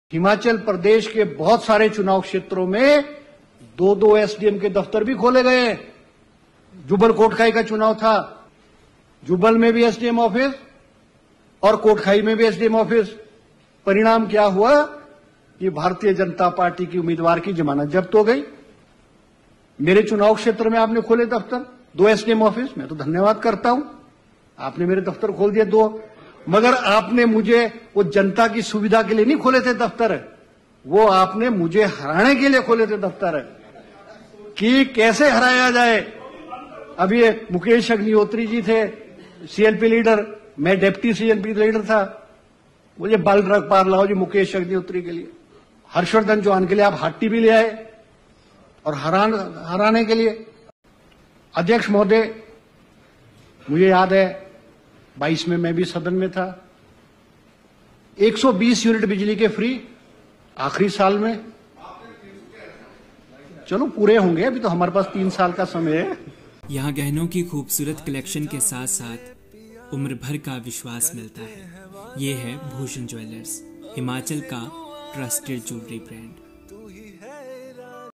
जब मंत्री ने गिरिपार को ST Status व शिलाई मे 2 SDM बिठाने को बताया उन्हें हराने की कोशिश
सिरमौर जिला के दूरदराज शिलाई हल्के के MLA एवं Industries Minister of Himachal Harshvardhan Chauhan का सदन में दिया गया यह भाषण उनके द्वारा दिल गए तर्कों के चलते चर्चा में है। मंत्री की मानें तो उनके विधानसभा क्षेत्र में पूर्व CM जयराम ने 2-2 SDM office सहित कई Offices जनता की मांग पर नहीं, बल्कि उन्हें हराने के लिए खोले थे। उन्होंने कहा कि, Giripaar को Schedule Tribe Status की 5 दशक पुरानी मांग मांग भी BJP ने उन्हें हराने के लिए पूरी की, मगर जनता ने फिर भी उन्हें जिताया। गौरतलब है कि, अपने करीबी Ex MLA बलदेव तोमर के कहने पर पूर्व मुख्यमंत्री जयराम ने Shillai में SDM, BDO, DSP व ExEn office सहित College, Hospital, उपतहसील व School आदि दर्जनों संस्थान खोल दिए थे और सिरमौर जिला के अन्य हल्कों के भाजपा नेता भी उस दौरान दबे स्वर में उनके यहां शिलाई जैसा विकास नहीं होने की बात कहते थे।